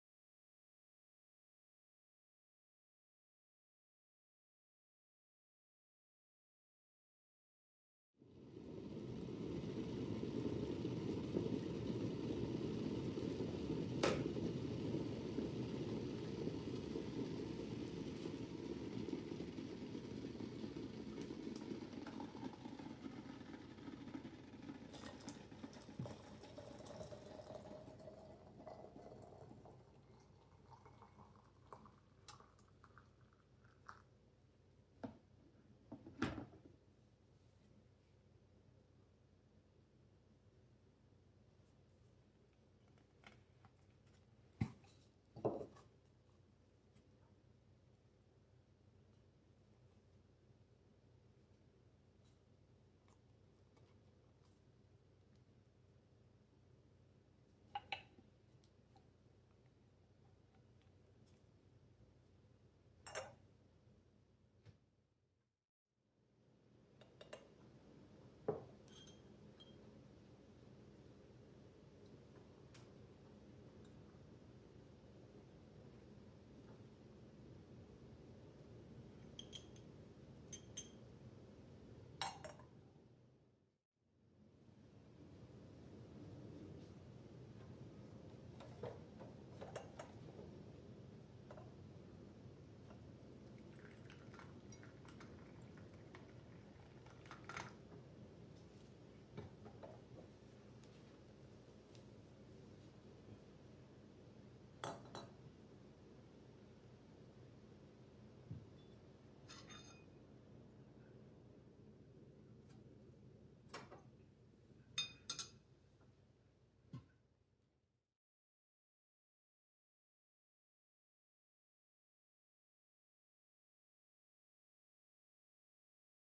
Afternoon Tea Time: October 20th!
Today's Tea Time included Vanilla Almond tea by The Republic of Tea in gorgeous and perfect-for-October vintage lusterware made in Japan! The teaware is so thin and I love that the teapot, creamer and sugar bowl resemble a pumpkin! I'm so happy that I employ the practice of filling my teapots with hot water to temper them against the boiling water needed to steep tea! This tea is so delicious and brings back memories of when Greg and I were first married and I used Lipton Orange Pekoe tea and Coffee Mate Vanilla Almond creamer! It was the beginning of an almost 33 year love affair with tea and tea time!!